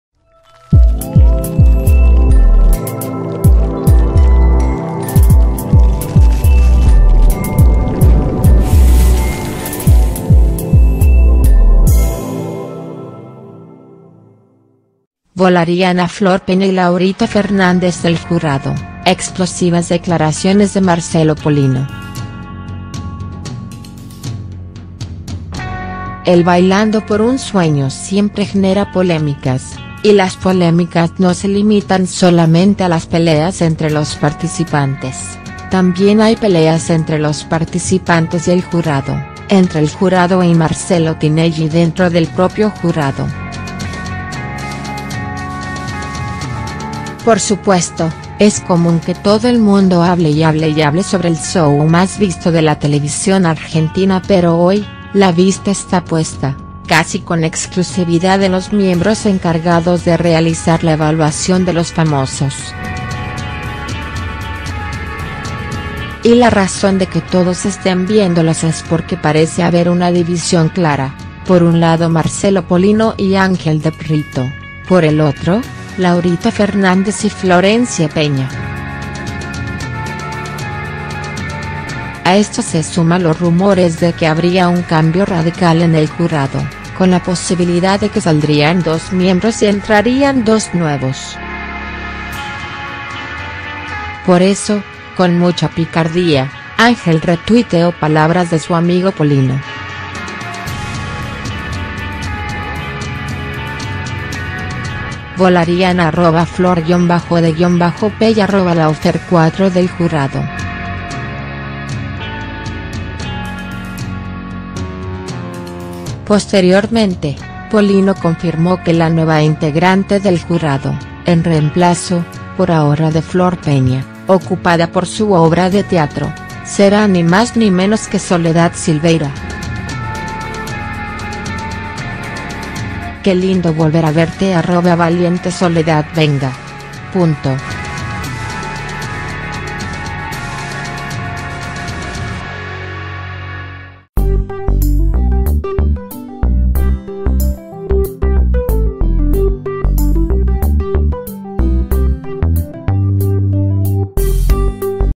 "Volarían a Flor Peña y Laurita Fernández del jurado": ¡Explosivas declaraciones de Marcelo Polino!
El periodista de espectáculos lanzó dos noticias realmente muy fuertes sobre el jurado y sorprendió a todo el mundo.